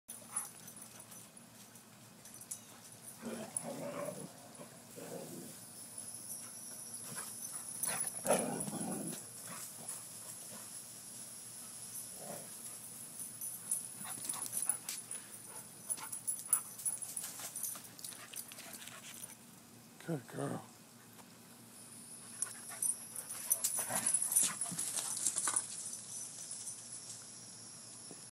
Berkeley and Willow become friends
Willow finally loosens up and has some fun with Berkeley